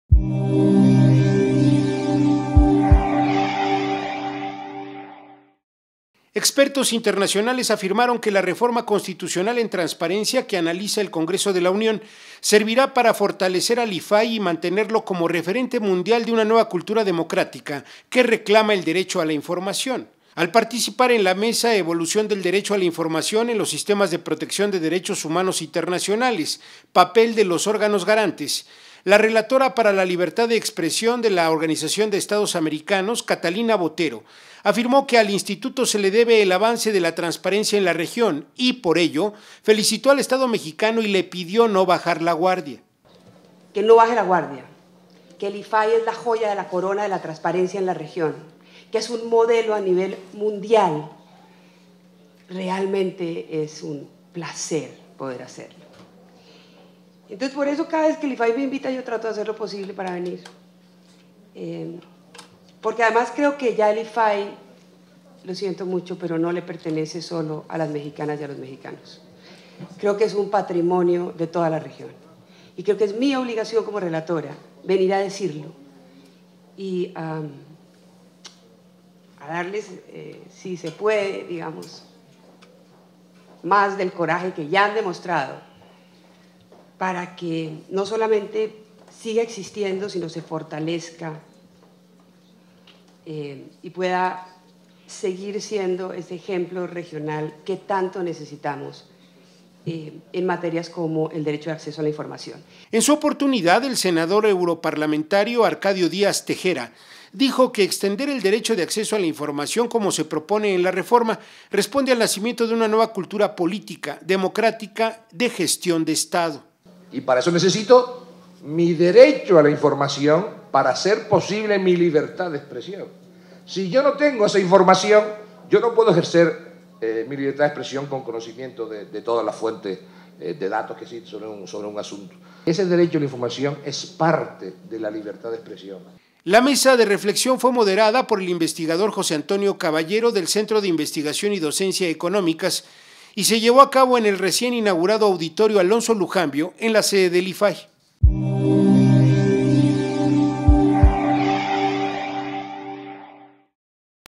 LLAMAN EXPERTOS INTERNACIONALES A FORTALECER AL IFAI
Expertos internacionales afirmaron que la reforma constitucional en transparencia que analiza el Congreso de la Unión servirá para fortalecer al Instituto Federal de Acceso a la Información y Protección de Datos (IFAI) y mantenerlo como referente mundial de una nueva cultura democrática que reclama el derecho a la información.